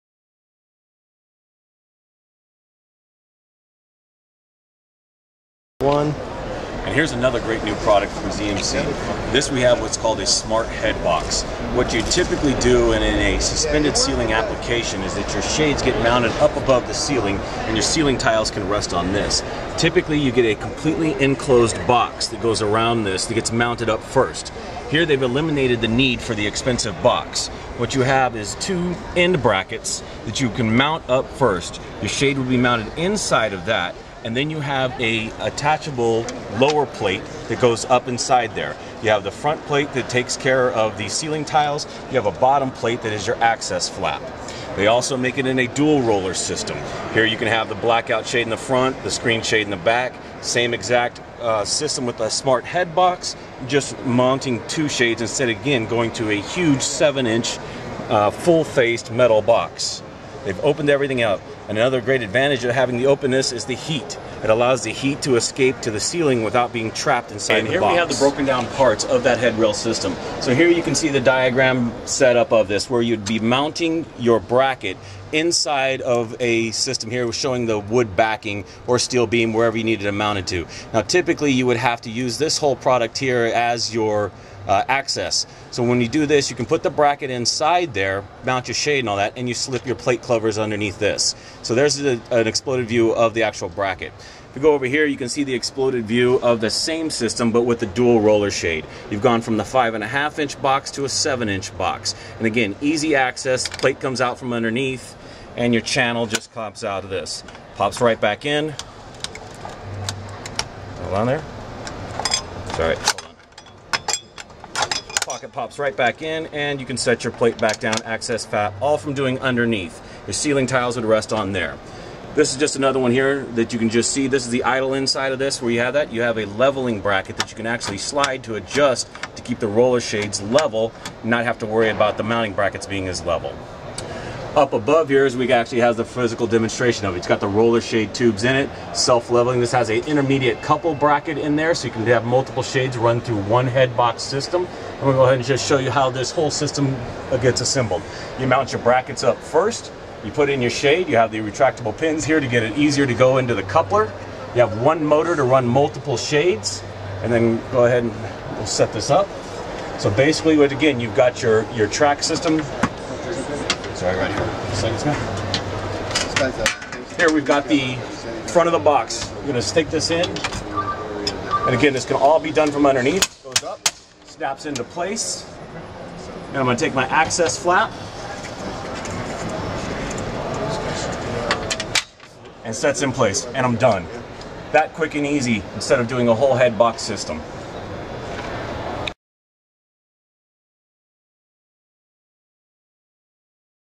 Low Cost Pocket Option with Access Flap and Ceiling Clip by 3 Blind Mice San Diego
This is a great low cost pocket option.  If you are looking at ceiling pocket for a commercial or residential project this is a great inexpensive way to go.  Instead of a large aluminum extrusion you have brackets instead.  The nice thing about this is it allows the heat to escape to the plenum which is usually an extra feature on expensive extruded pocket material.

The other unique thing is it has a ceiling clip so you can use this to support suspended ceiling or you can do it without the lip and butt drywall right up to it. This creates a nice clean look for your shades and it gives a very professional look and increases your view.

If you want a professional installation of this pocket call 3 Blind Mice Window Coverings.